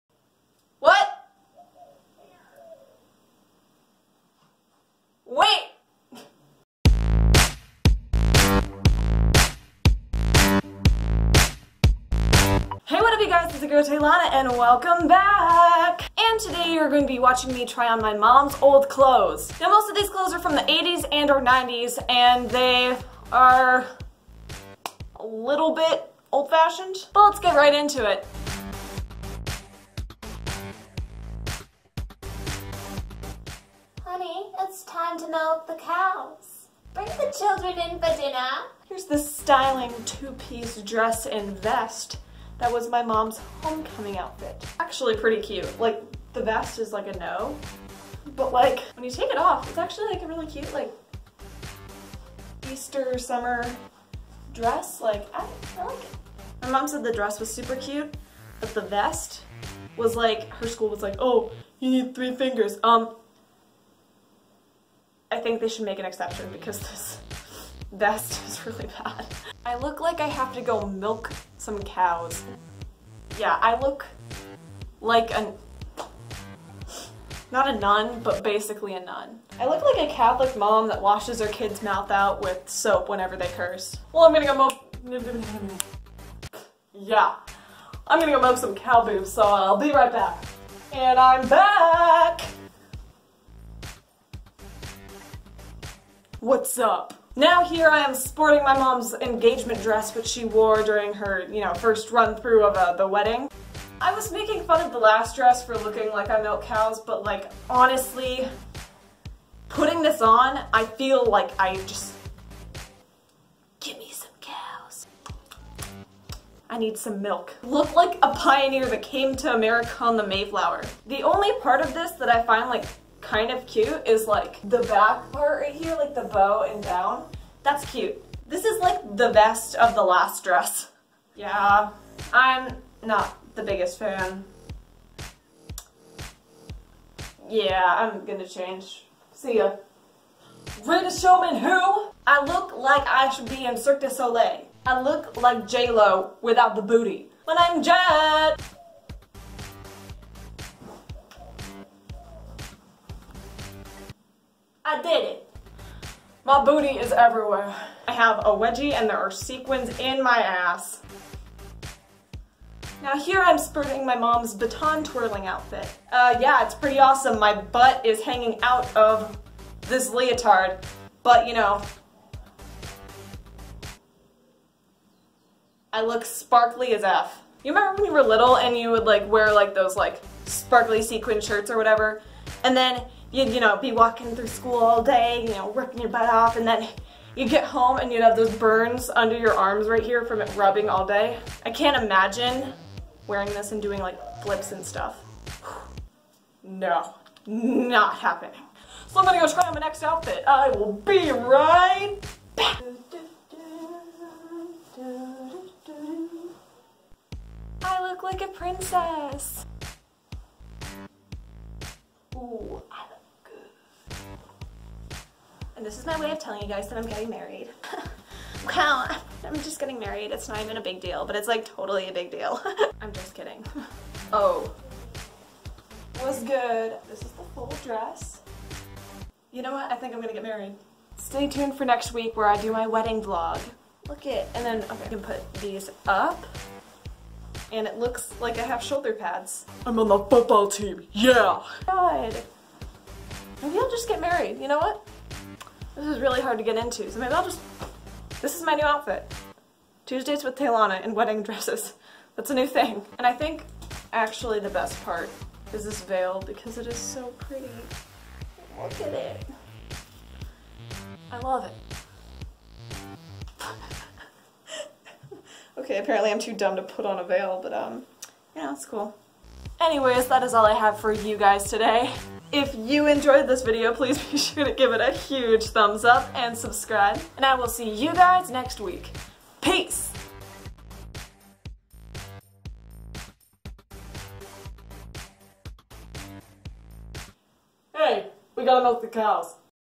TRYING ON MY MOM'S OLD CLOTHES!!
I try on my mom's old clothes! SPECIAL OUTFIT AT THE END!! This was a little different. But at least I look goooood!